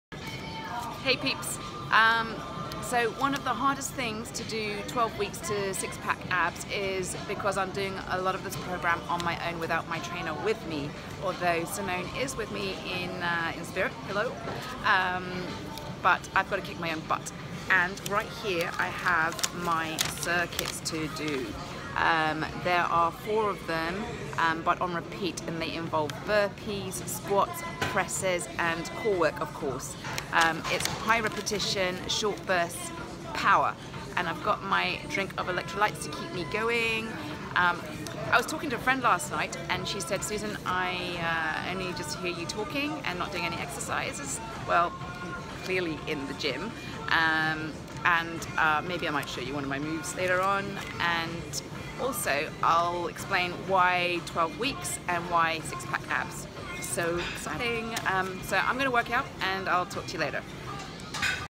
12 Weeks to 6 Pack Abs
Just in the gym to start some HIIT